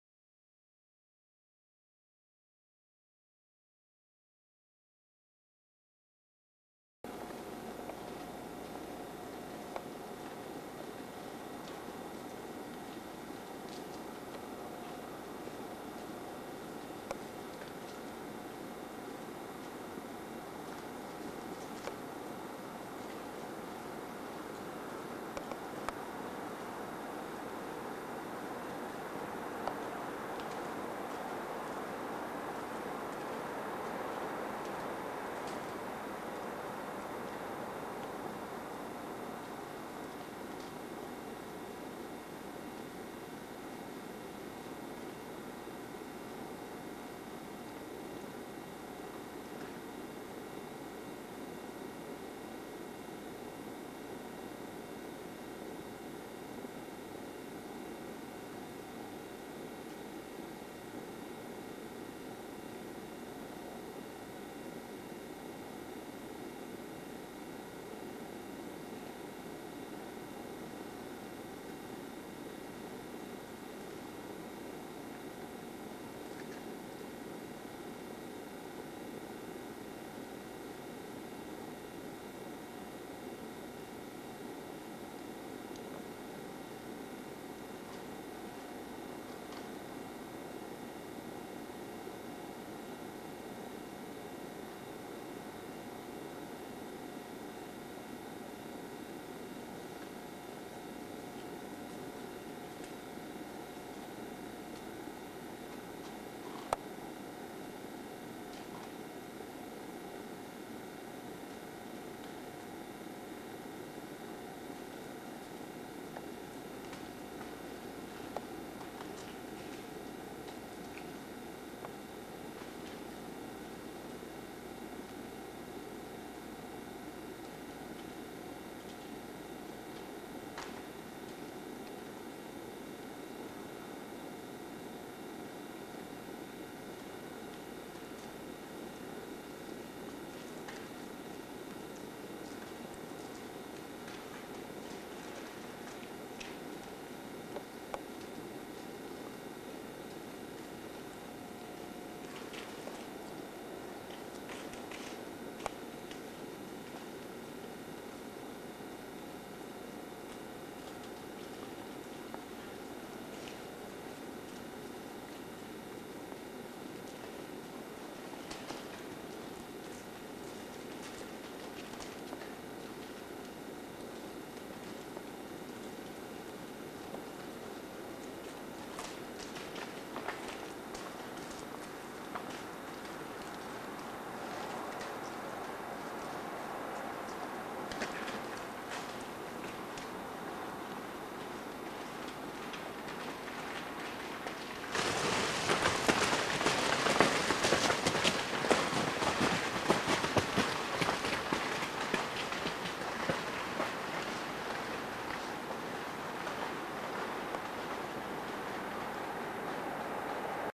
A Deer Stampede.wmv
On a cold day in December, I sat in my stand and video taped a long string of does as they nervously walked across a noisy, icy snow cover. They finally lose their nerve and change their minds about the direction they want to go.